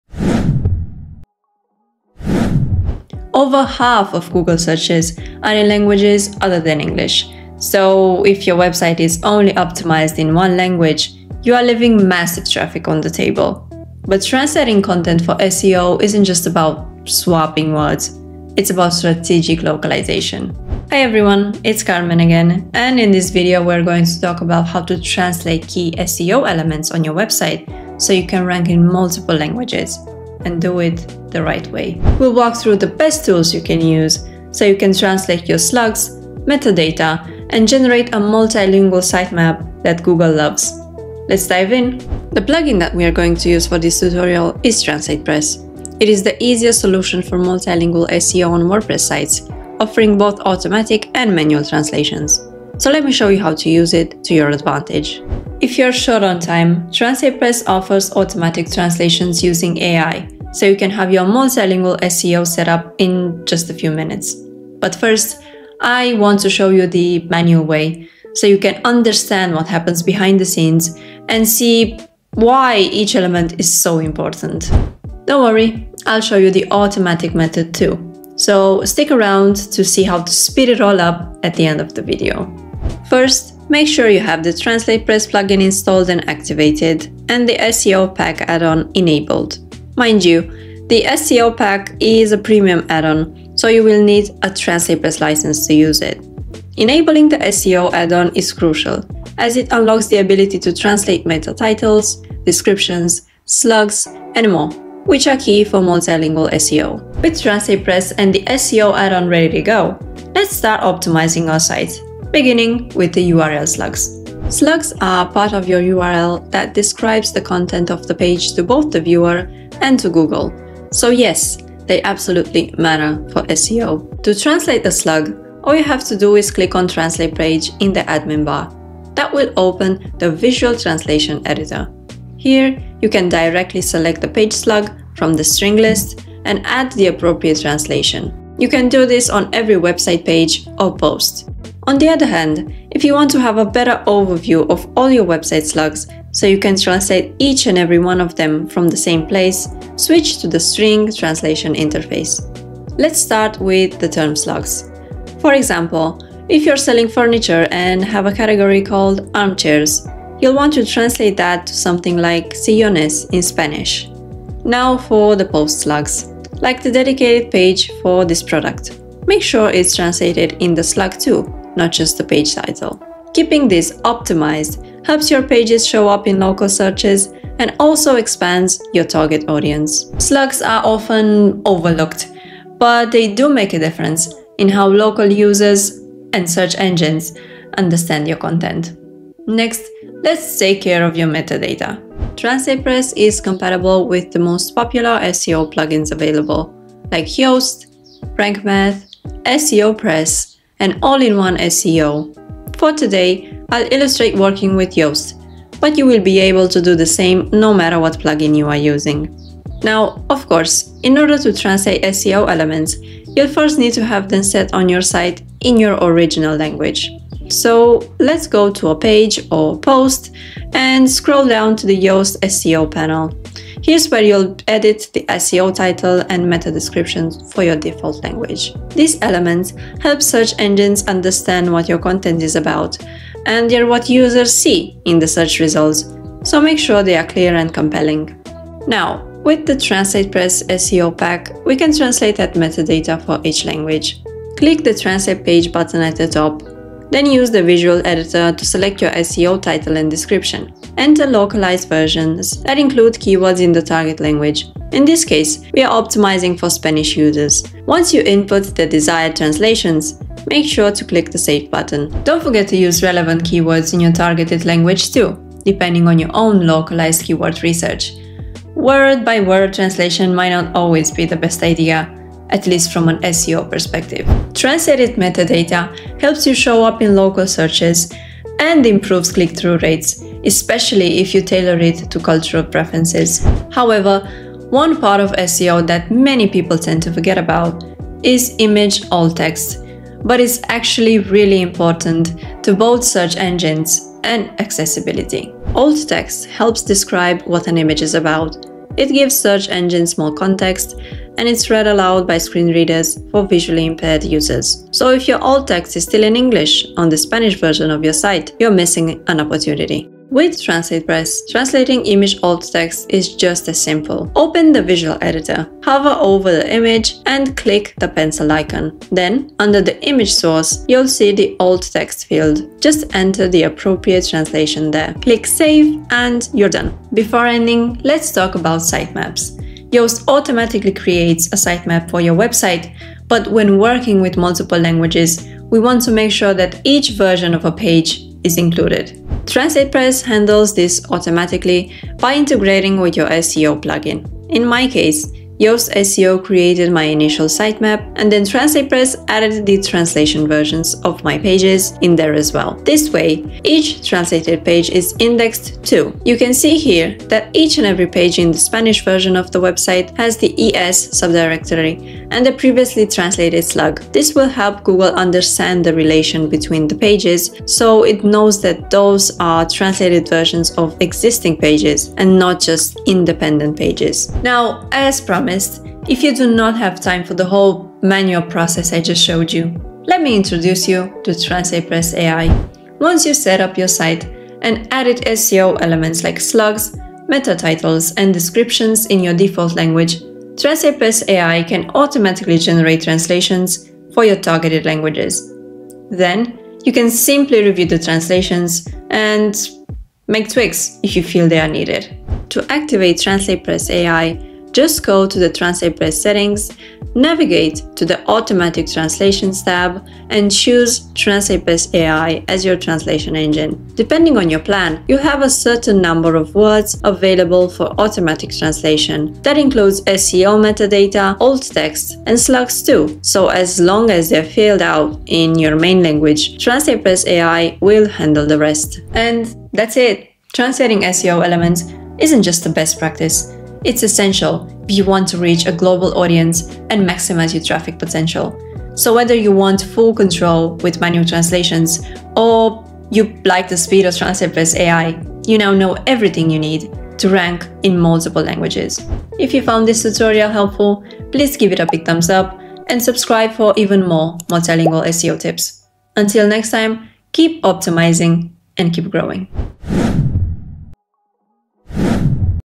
Grow Your Website Traffic with Multilingual SEO: Learn to Translate SEO Elements
Want to rank your website in multiple languages and grow global traffic without recreating your content from scratch? In this tutorial, I’ll show you exactly how to translate SEO elements on your WordPress website using both the manual and automatic methods.

➡️ Join our Facebook TranslatePress Community and get support from others just like you

Chapters:

00:00 Intro
01:23 Install TranslatePress & Enable SEO Pack Add-on
01:47 Translate URL Slugs
03:09 SEO Plugin Integration
03:39 Translate SEO Metadata
04:43 Translate Alt Text
05:27 Generate Multilingual Sitemap
06:14 Automatic SEO Translation with TranslatePress AI